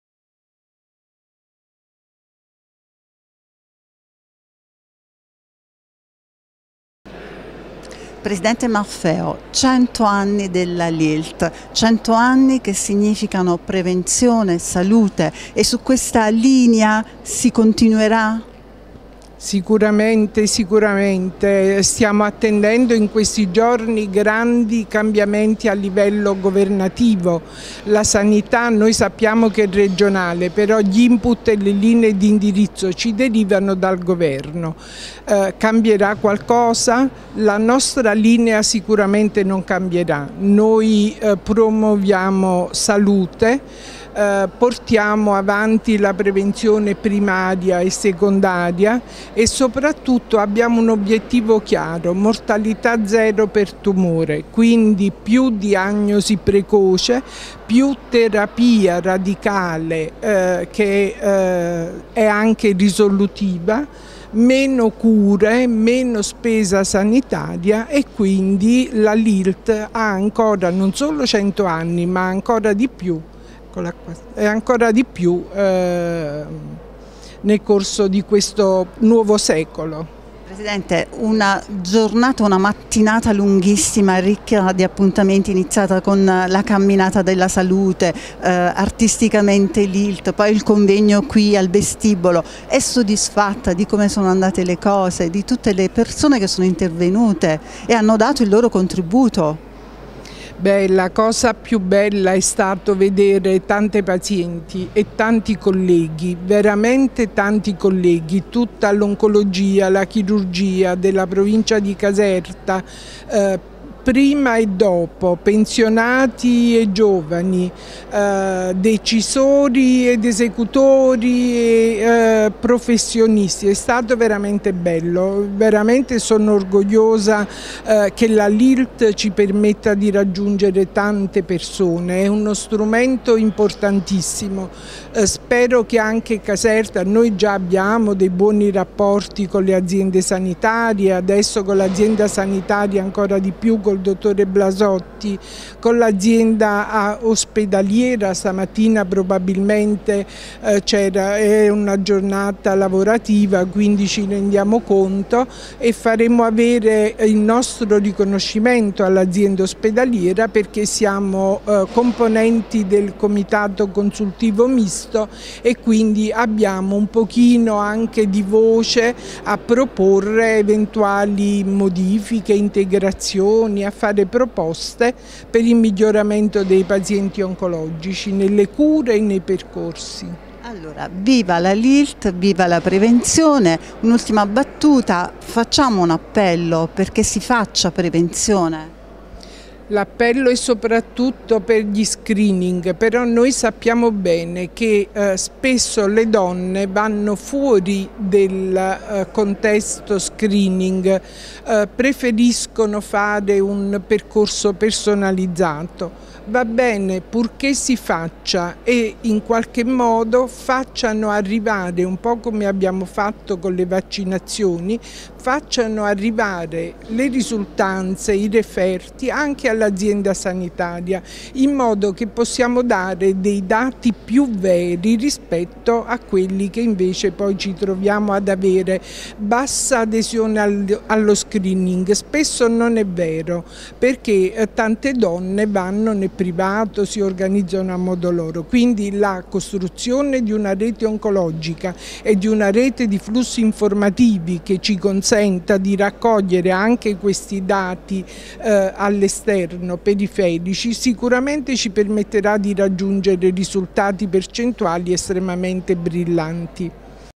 Cento anni della Lilt. Angela Maffeo, presidente Lilt Caserta
Celebrati alla Reggia di Caserta i Cento anni della Lilt nel segno della prevenzione. Angela Maffeo, Lilt Caserta per @ondawebtvCE